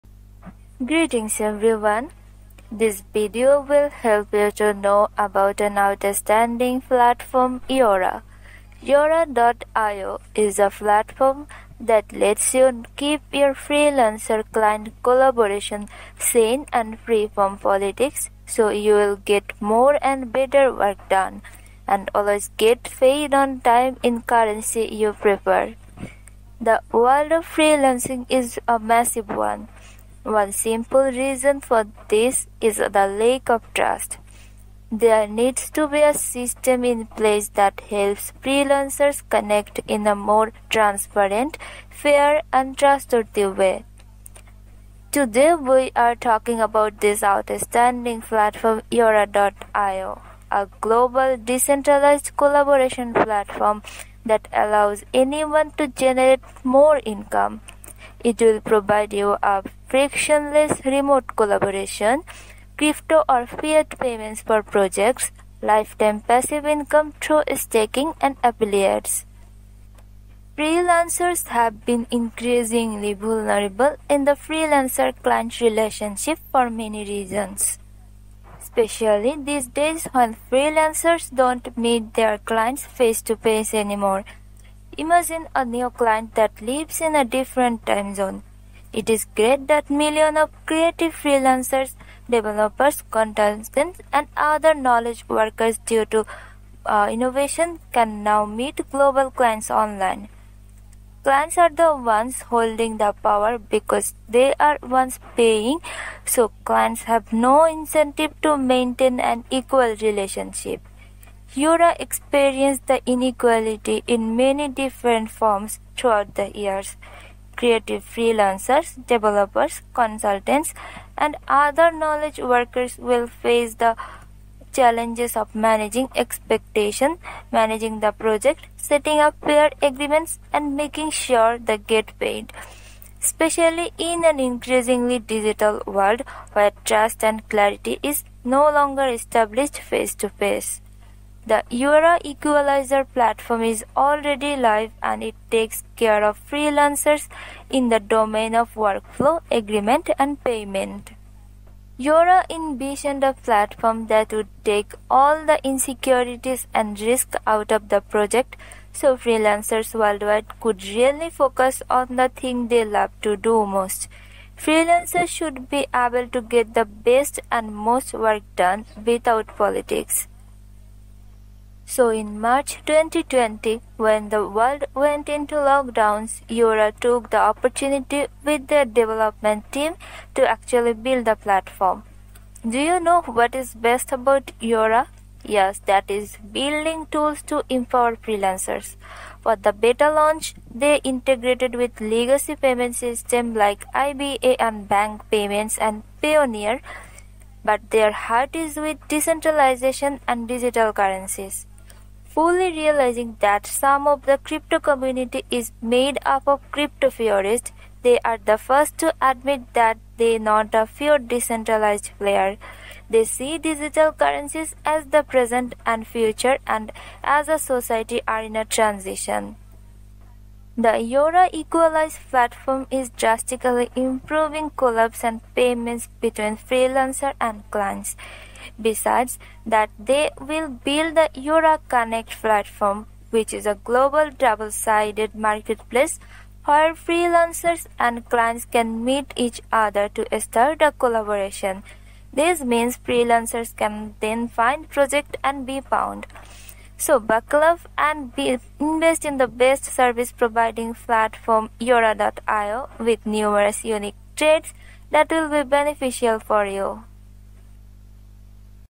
YURA EQUALIZE is drastically improving collabs and payments between freelancers and clients
The Yura ecosystem is based on the blockchain and allows for secure, trustless transactions between clients and freelancers. The YUR token is used as the core of this ecosystem.  The token is ERC20-compliant and will be tradable on an open market. Yura was developed in order to address the most burning issues of freelancers working in today’s gig economy, including.

Author information:

ERC-20 Wallet Address: 0x34E5e9749ceB185e29cd239EA631324dD03Bf90A